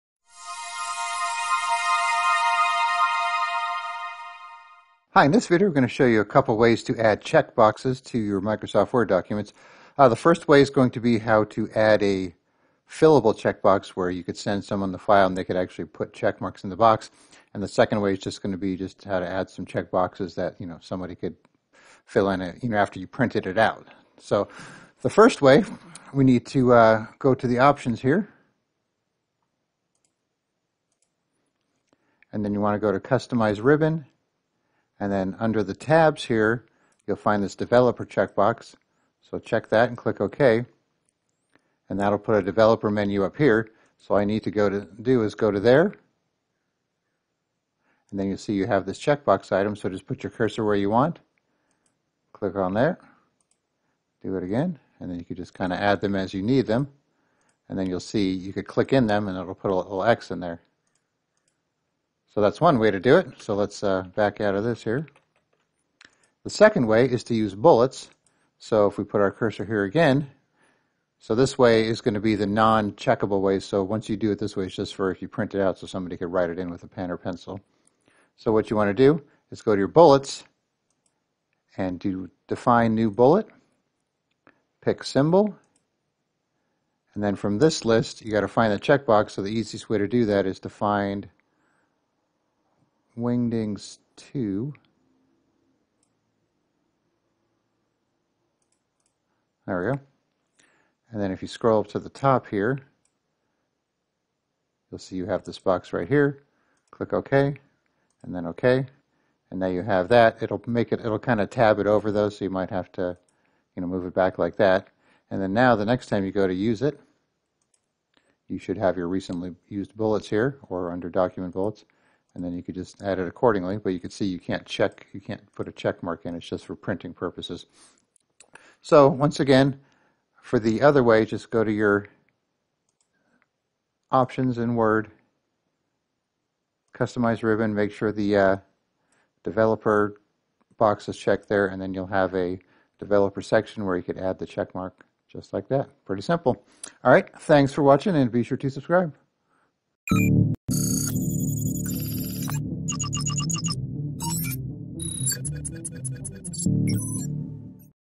How to Add Fillable Checkboxes to Microsoft Word Documents
If you need to add a fillable checkbox to a Word document so you can send it out and have someone else fill in the boxes as needed, this is a very easy process to do. We will also be showing you how to add non-fillable checkboxes that can be used for printed copies if you do not want them filling in the document itself or if you want to keep it more read only.

Check out our book titled Word for Seniors Made Easy to take your Windows skills to the next level! And it's not just for seniors!